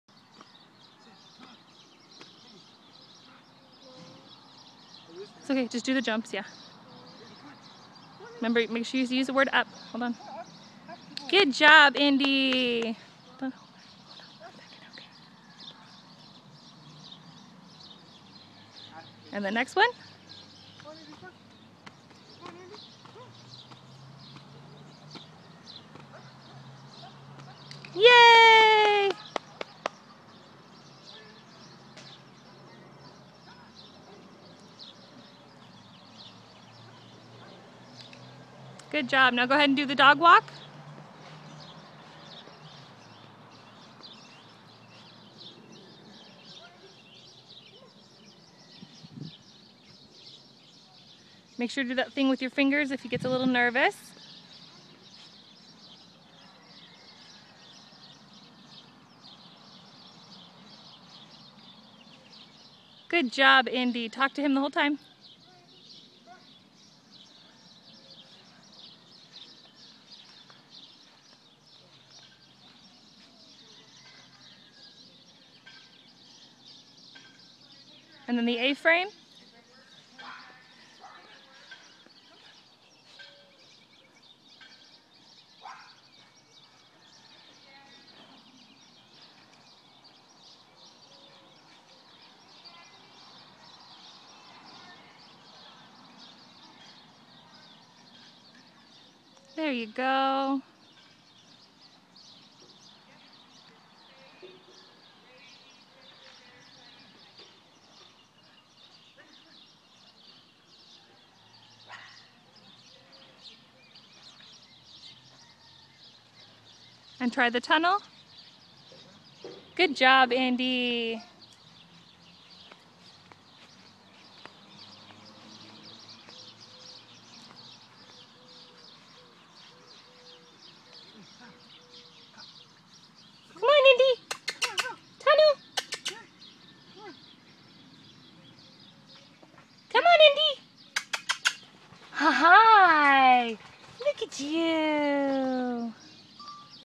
Clyde learning Agility
Clyde is available for adoption through the Pet Rescue Center (please ignore the fact I called him Indie-that's his clone lol) and this is his first your around the agility field! Look how eager he is to learn!!!